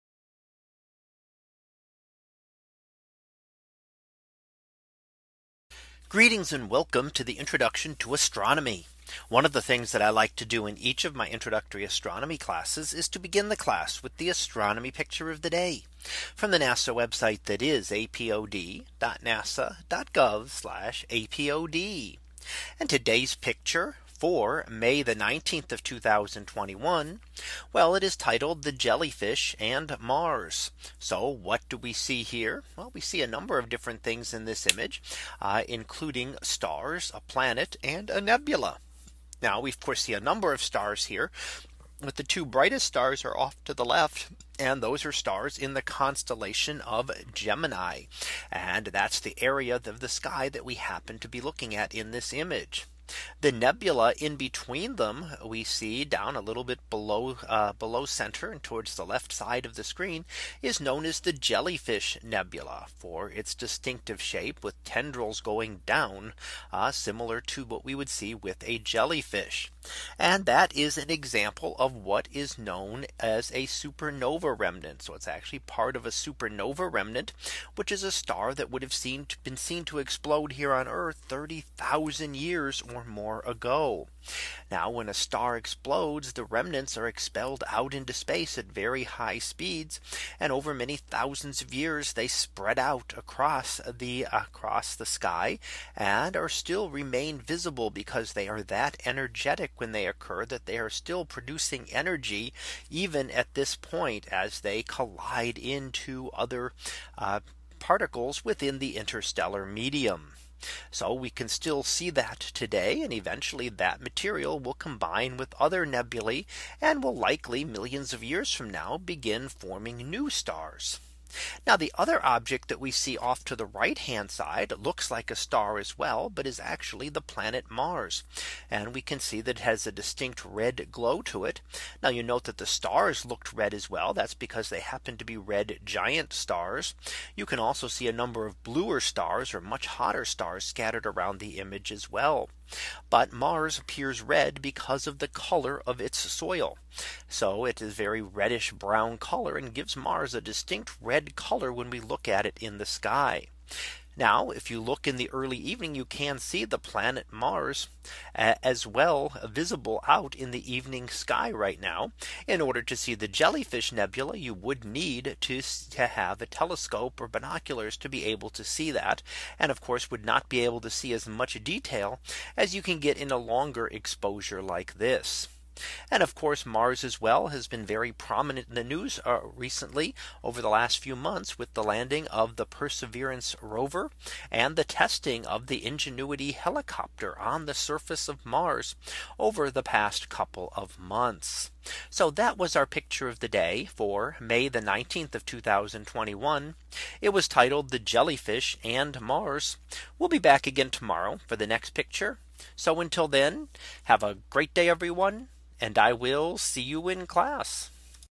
2021 May 19 - The Jellyfish and Mars
In today's image, we see The Jellyfish Nebula, a supernova remnant from a star which exploded over 30,000 years ago. We also see the planet Mars - much closer - and where the Perseverance Rover and Ingenuity Helicopter are currently exploring.